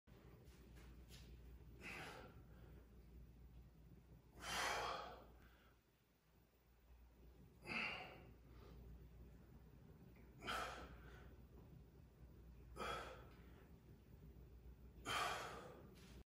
Standing Thoracic Rotation
Wonderful stretch and warm up for the mid to upper back